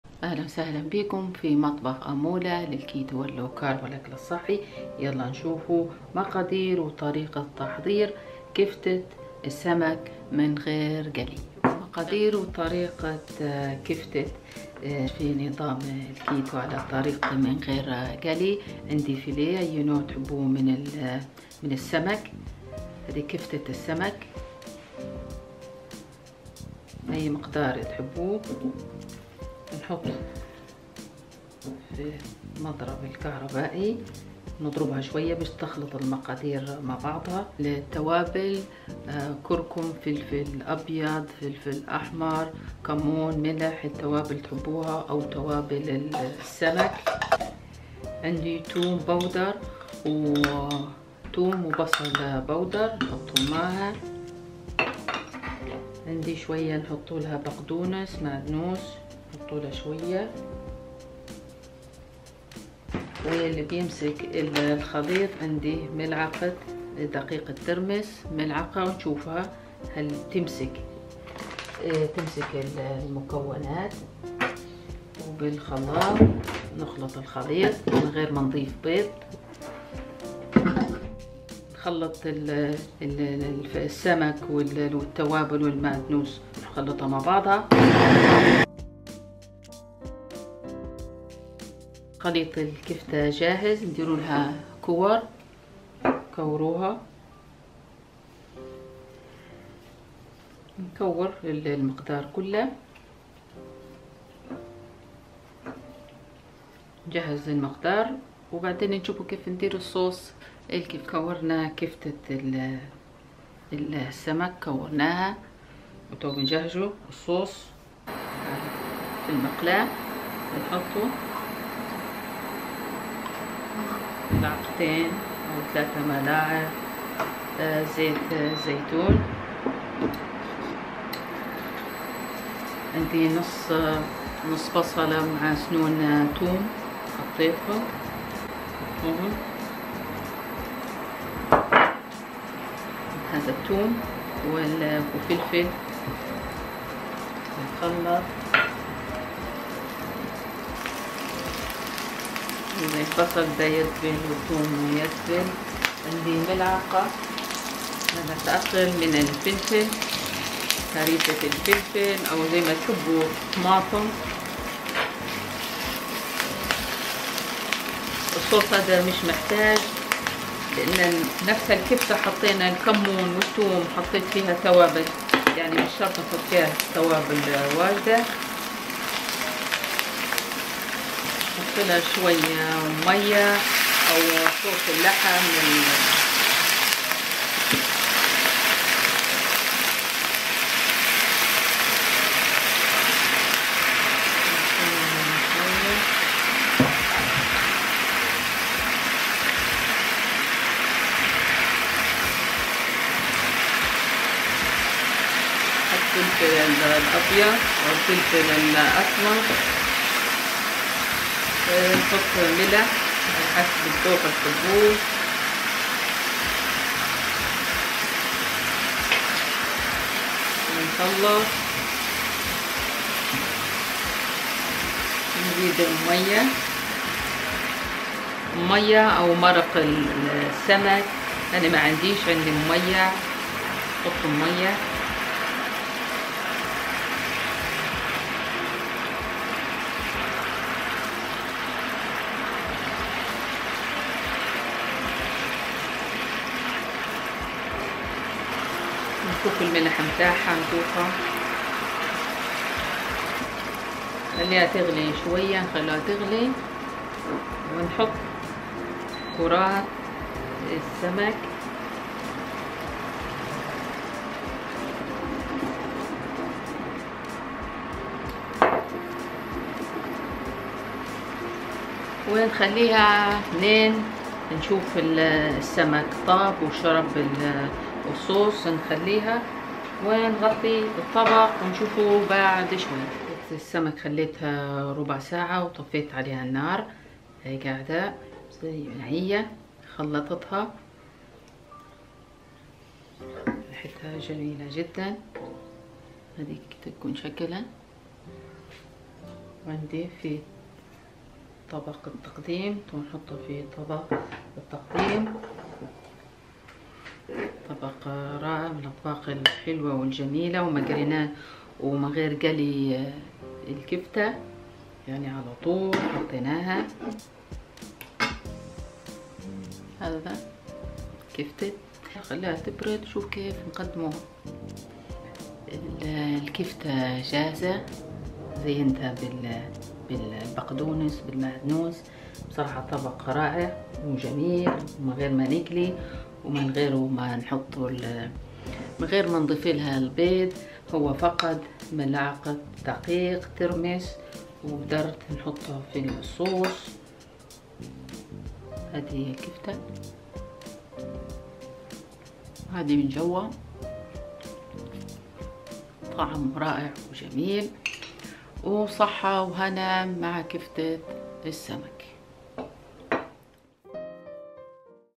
كرات كفته سمك كيتو و لو كارب من غير قلي او اضافه بيض وجبه سهله و سريعه تحضير
كرات سمك او كفته سمك خالي من جلوتين وجبه كامله مع الخبز و سلطه